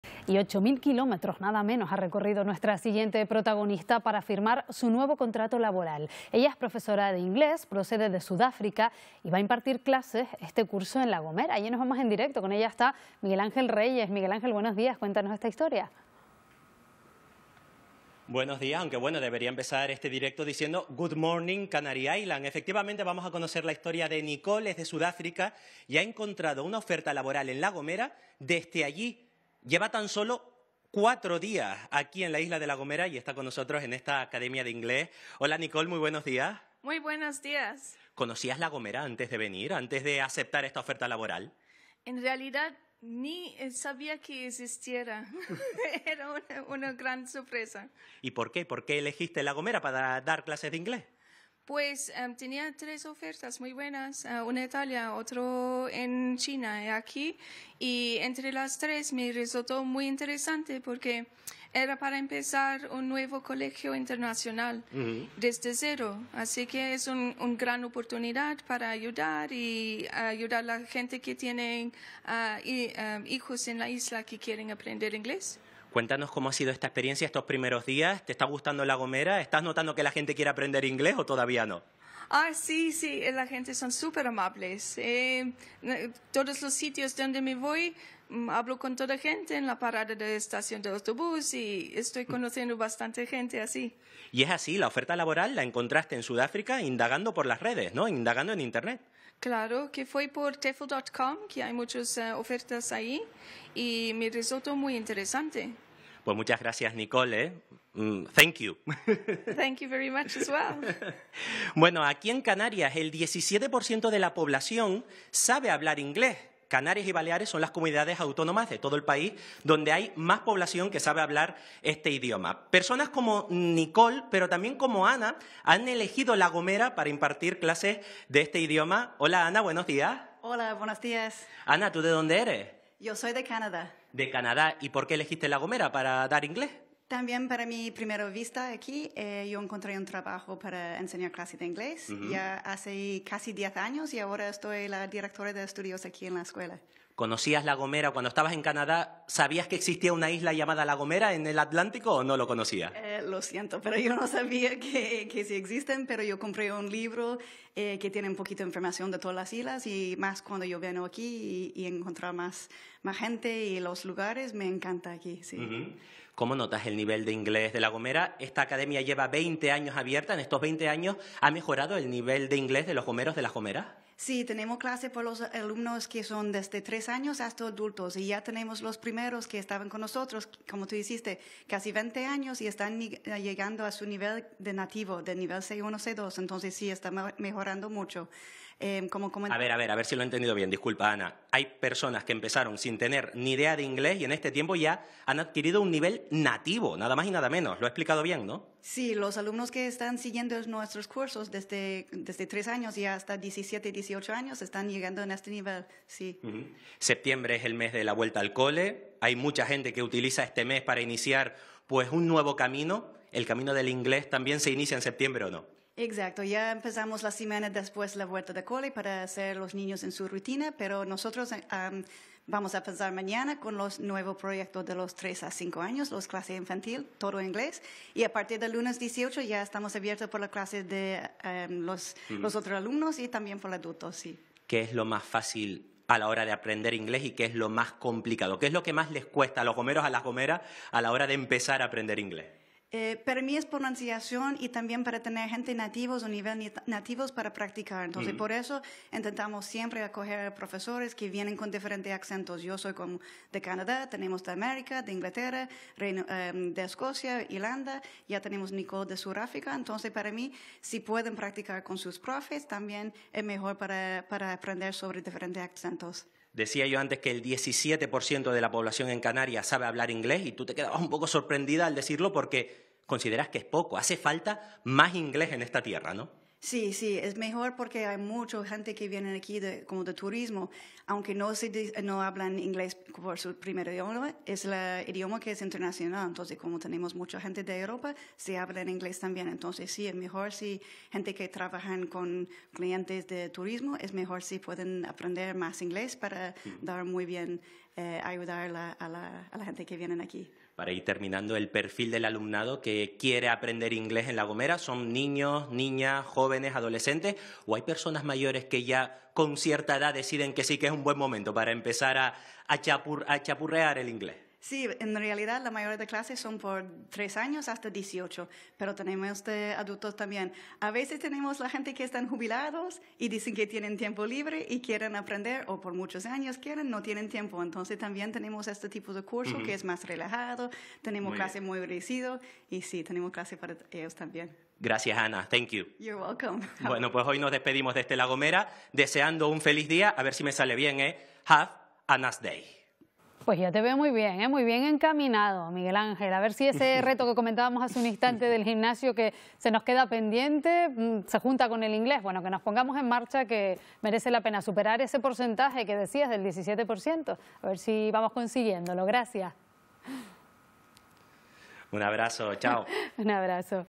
Desde Sudáfrica a La Gomera
Es la historia de Nicole Pia Cabral. 8 mil kilómetros ha recorrido para firmar su nuevo contrato laboral. Es profesora de inglés e impartirá clases este curso en La Gomera.